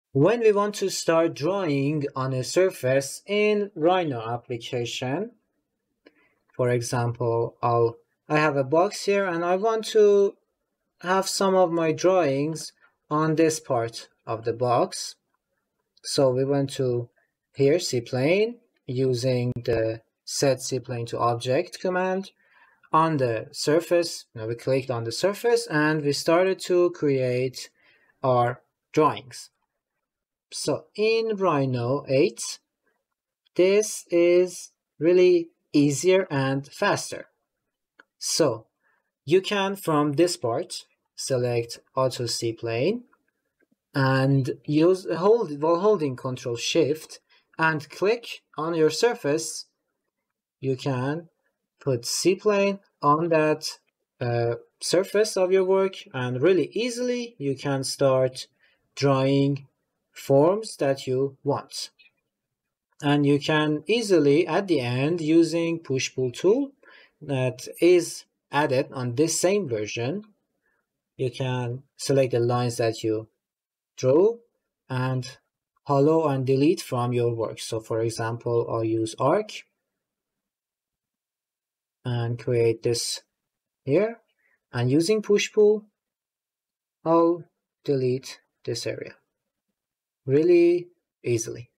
🚀 𝐑𝐡𝐢𝐧𝐨𝐜𝐞𝐫𝐨𝐬-𝐍𝐞𝐰 𝐅𝐞𝐚𝐭𝐮𝐫𝐞 𝐀𝐮𝐭𝐨𝐜𝐩𝐥𝐚𝐧𝐞 𝐢𝐧 𝐑𝐡𝐢𝐧𝐨 𝟖 - 𝐄𝐧𝐡𝐚𝐧𝐜𝐢𝐧𝐠 𝐘𝐨𝐮𝐫 𝟑𝐃 𝐌𝐨𝐝𝐞𝐥𝐢𝐧𝐠 𝐖𝐨𝐫𝐤𝐟𝐥𝐨𝐰 💡
In this video, you will learn how this powerful tool revolutionizes your 3D modeling experience, simplifying the process of creating complex surfaces and enhancing design precision. 🔧

 Join us as we delve into the capabilities of Autocplane and learn how to leverage its functionalities to streamline your architectural and engineering projects. 🏗️

 Stay ahead in the world of 3D design with Rhino 8's Autocplane and take your creations to new heights! 📐

Amir Karami